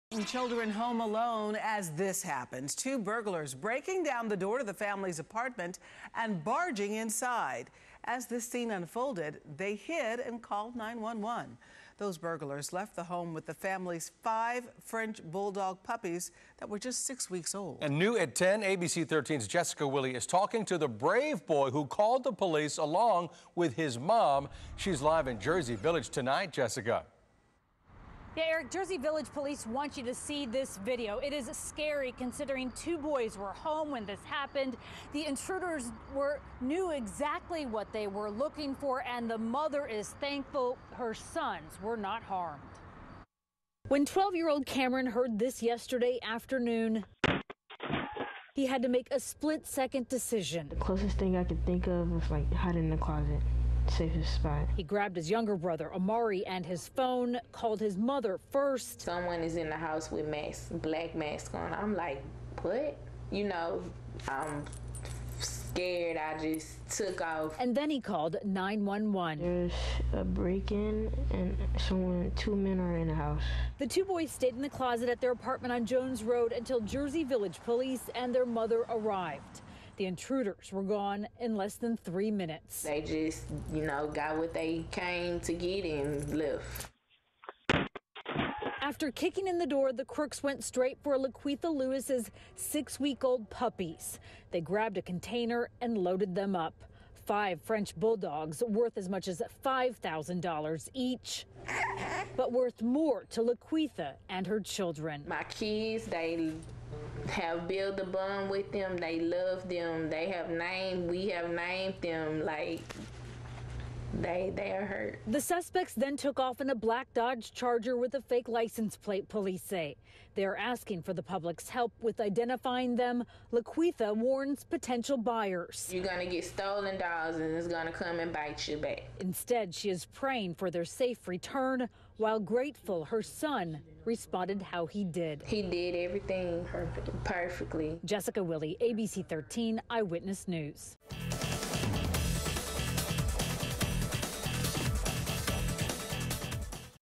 French bulldog puppies taken during home invasion, police say
A juvenile was in the apartment at the time and called 911 while in hiding. A video shows the suspects kicking in the door and taking the puppies.